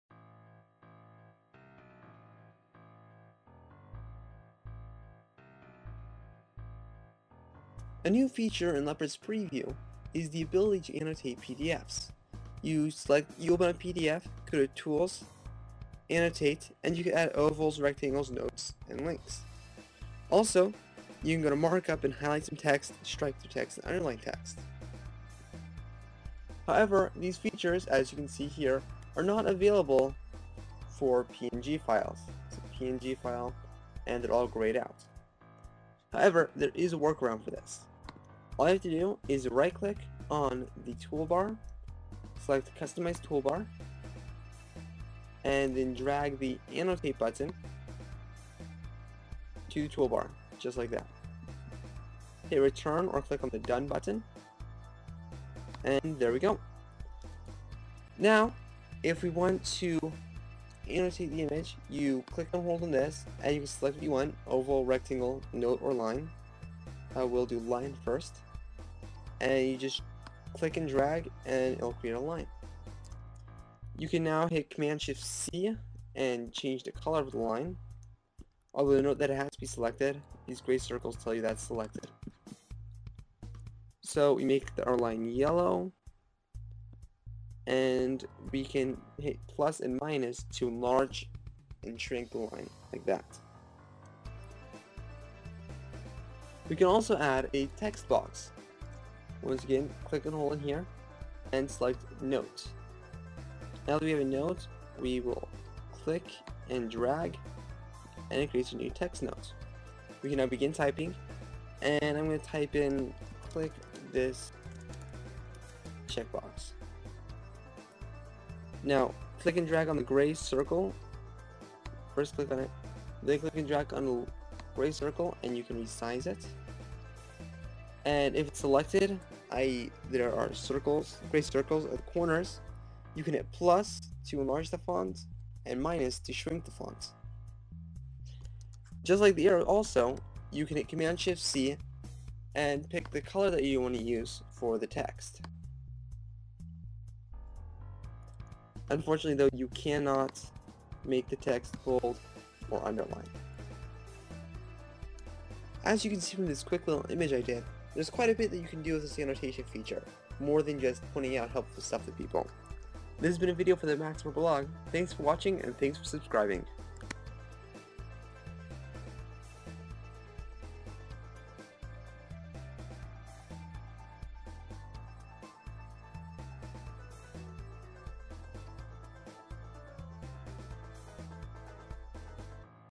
Annotate Images in Leopard's Preview
There's a neat little trick to let you draw on your images in Preview.